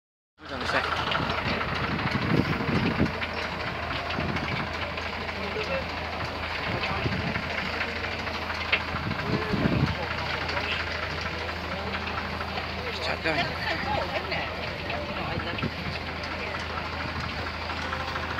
Aveling and Porter Road Locomotive 'Flame Lilly' at the Wdvt&sec annual rally 2019
At the wdvtsec annual rally 2019